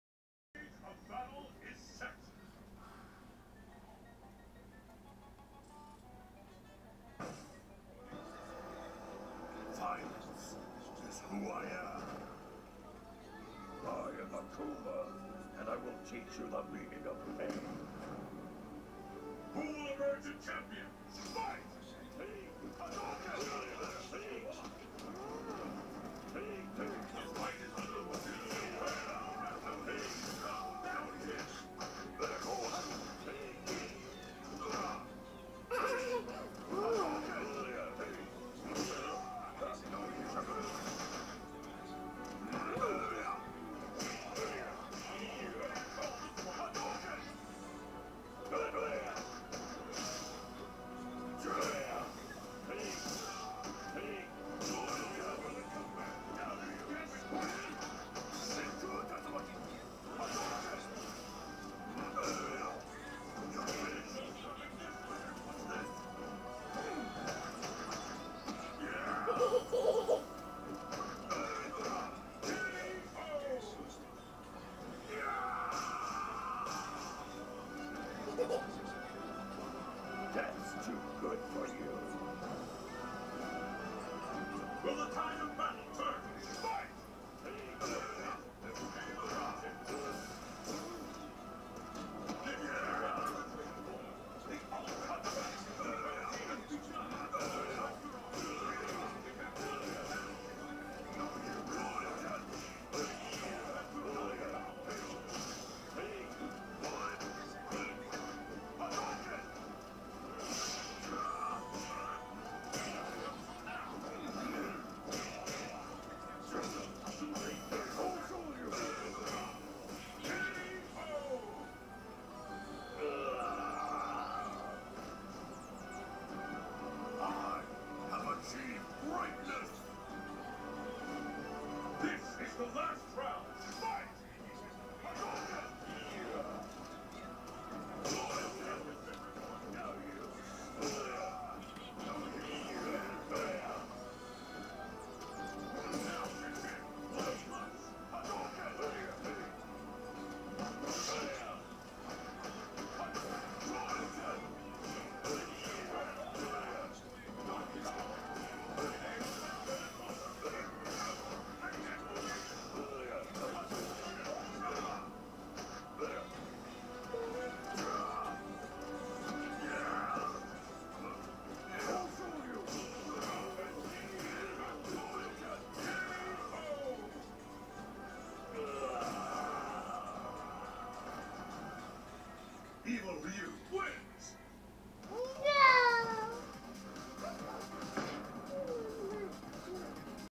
Nashnyc vs xWAx Solid
testing HD quality on a camera i have. an endless lobby before NGB pyscho kicked me out of his endess lobby. GGs to solid